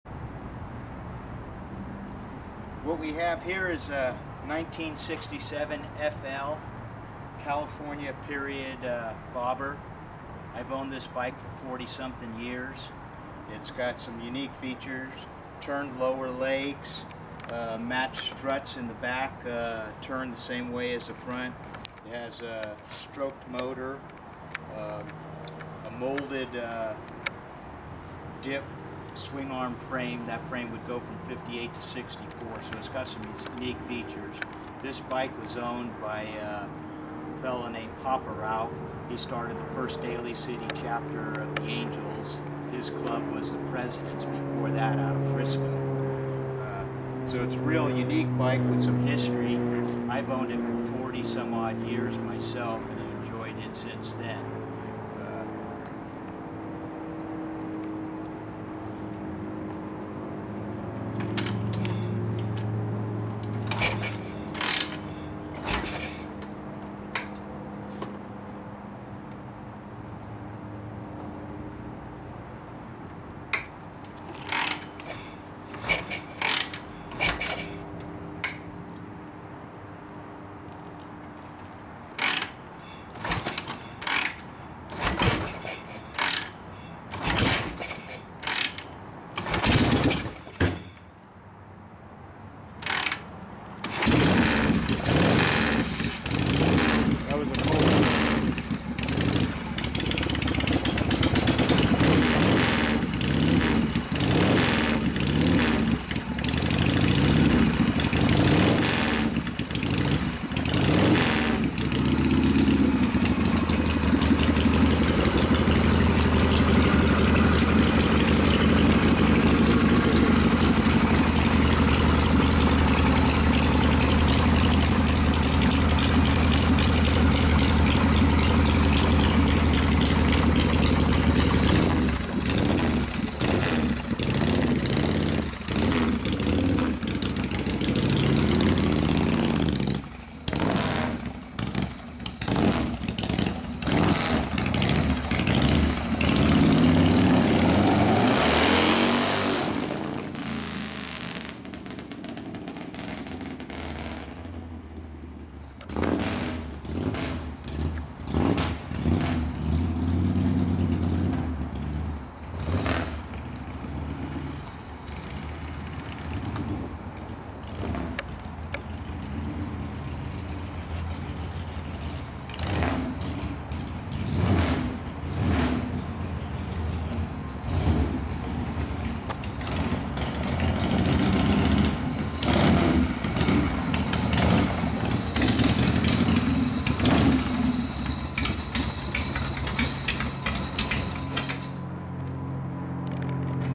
Hunting Harley's, generator shovel bobber
Todd's friend Vince describes and rides a real old school generator Shovel bobber built over 40 years ago, for pix, go